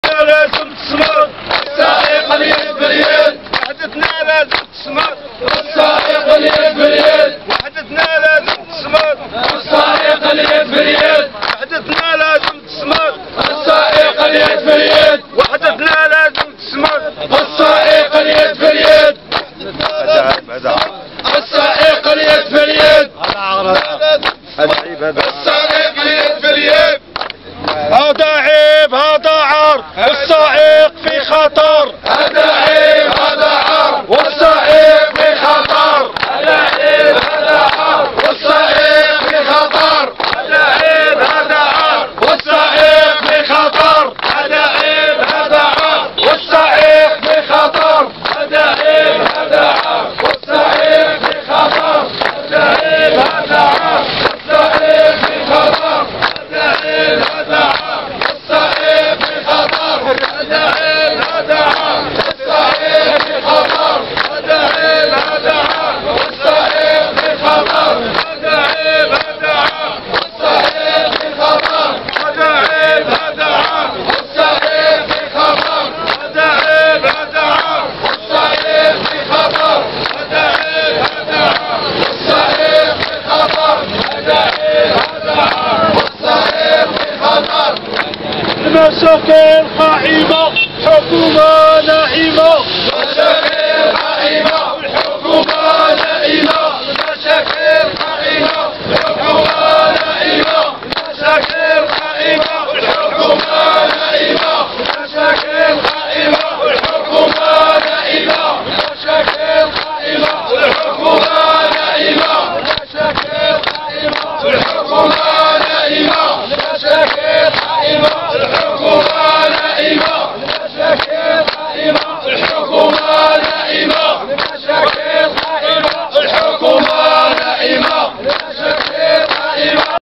اضراب انذاري سيارات الاجرة الكبيرة النقل مزدوج ب:ف-ب-صالح
بيان قطاع مهني سيارات الاجرة:1-نعم للوحدة الترابية
2-لا للتدخل الاجنبي في السيادة المغربية
3-نعم لتطبيق العقد النموذجي
4-نعم لمحتوى القرار الوزاري
5-لا لسحب رخصة السياقة لانها مصدر عيش السائقين
6-لا للشواهد الطبية المبالغ فيها
7-إلغاء الأحكام القضائية الصادرة لبعض المأذونيات